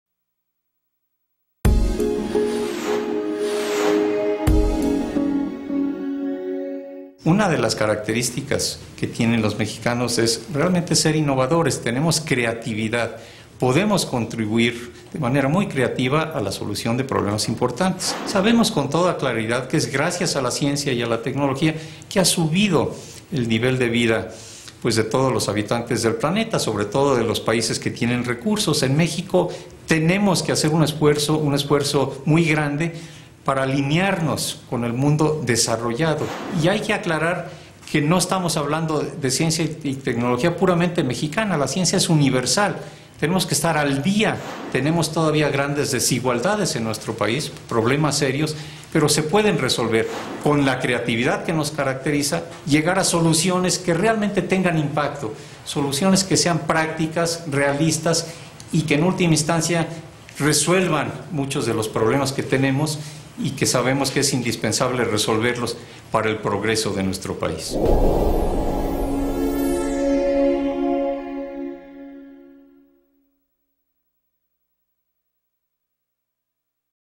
Mario Molina: la ciencia y la solución a problemas prácticos
El Premio Nobel de Química Mario Molina, entrevistado por Aura Branding para el lanzamiento de México como Marca País, habla sobre innovación y creatividad en la solución de problemas